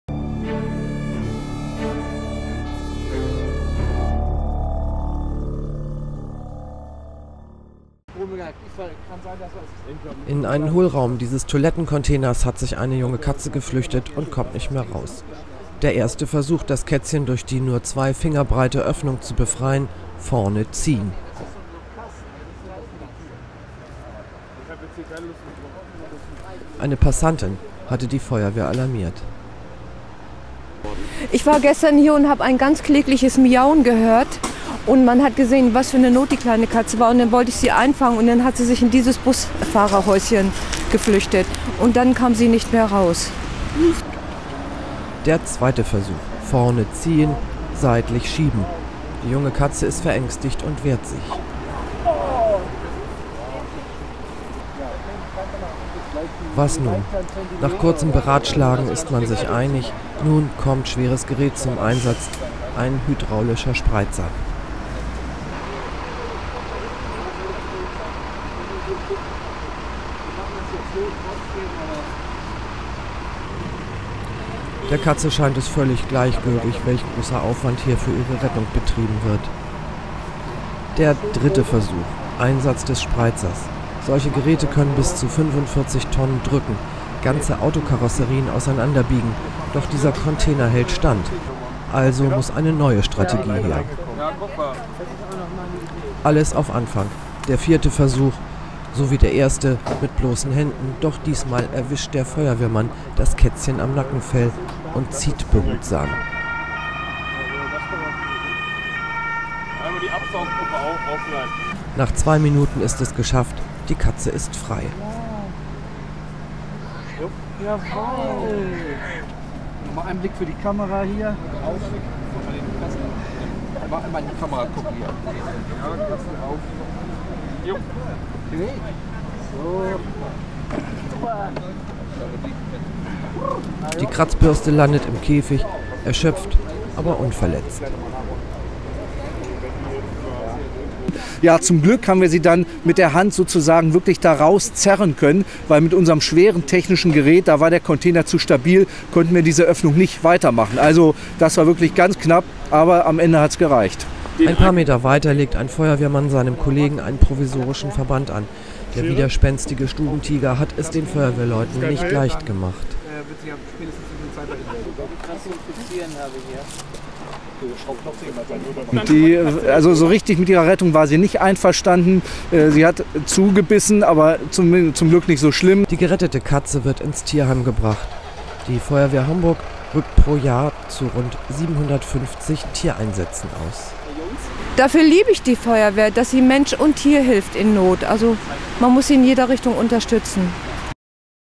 Feuerwehr befreit kleine Katze
In der Eckaufhängung eines Sanitätscontainers saß die kleine Katze fest. Nach ersten erfolglosen Versuchen die Katze per Hand aus der Aufhängung zu befreien, griffen die Einsatzkräfte der FF Lokstedt zu schwerem Gerät. Ein hydraulischer Spreizer sollte die Öffnung vergrößern und dem Kätzchen zur Flucht verhelfen. Im normalem Einsatz wird der Spreizer genutzt um Zugang zu Pkw-Insassen zu bekommen, die nach einem Unfall in ihrem Fahrzeug eingeklemmt sind. Doch die verstärkte Containeraufhängung war nicht so leicht zu biegen wie das Autoblech. Also setzten die Feuerwehrmänner wieder auf Handarbeit. So ganz schien das Kätzchen übrigens nicht mit seiner Rettung einverstanden. Einen ihrer Helfer biss die Katze in den Finger.